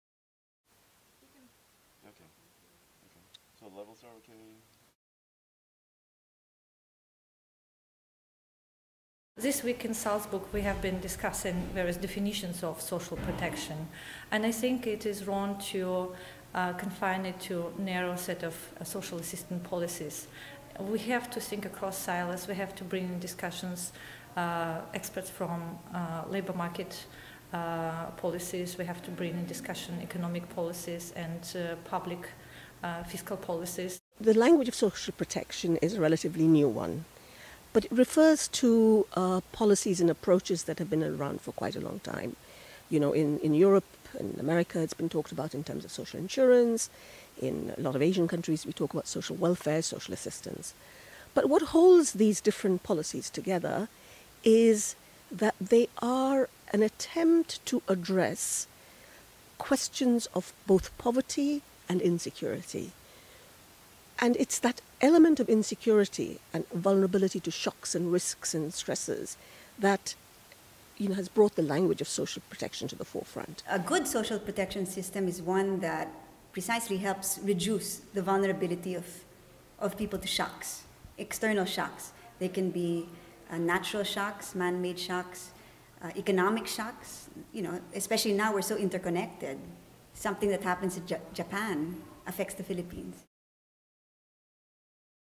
Why is social protection so important? - Tatsiana Lintouskaya, Naila Kabeer and Toby Monsod
Why is social protection so important? Where in the world you find the answer?

Tatsiana Lintouskaya (Program Director, Salzburg Global Seminar), Naila Kabeer (Professor of Development Studies, London University) and Toby Monsod (Coordinator of the Philippine Human Development Network) analyse the role of social protection and why it is important, especially now that we live in an interconnected world where a shock that affects a particular country has repercussions on diverse aspects of many other entities.

Salzburg Global Seminar
Session 483: Economic Growth and Social Protection in Asia
7 - 12 November, 2011